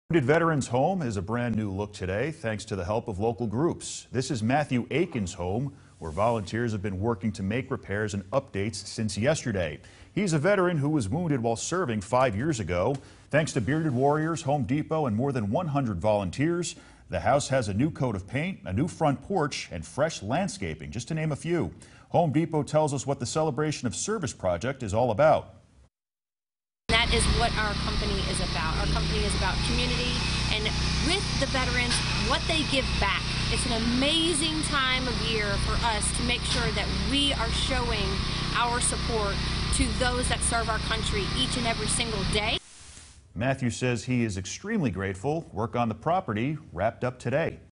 Wounded veteran's home has a brand new look
A wounded veteran's home has a brand new look. Thanks to Bearded Warriors, Home Depot, and more than 100 volunteers, the house has a new coat of paint, a new front porch, fresh landscaping, and more.